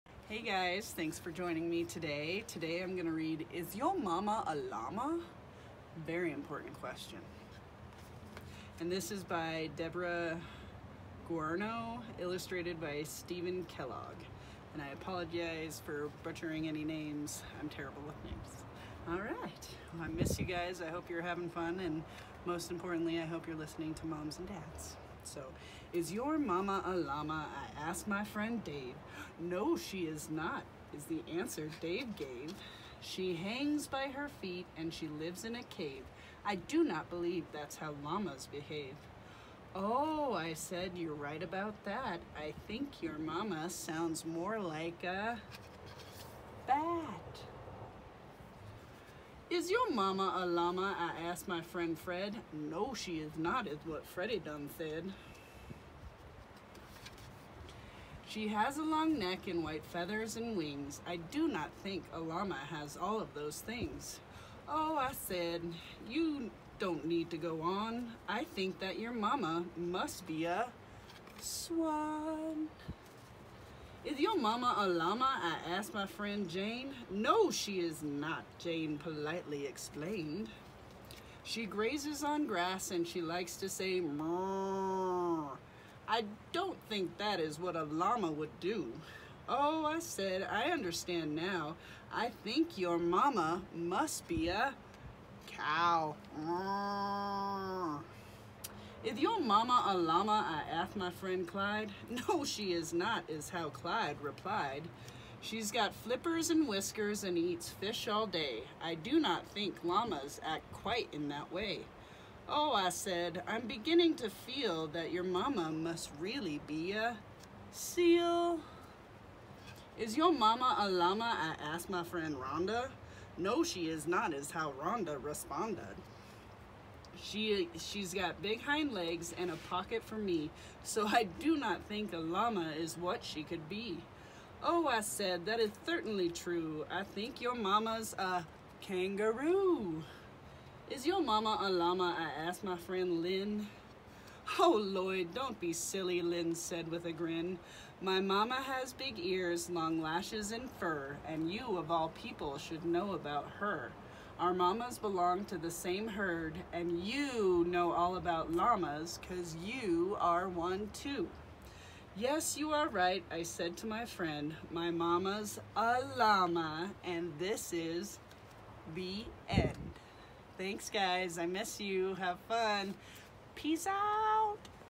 Ms. Megan reads Is Your Mama a Llama? By Deborah Guarino
Illustrated by Steven Kellog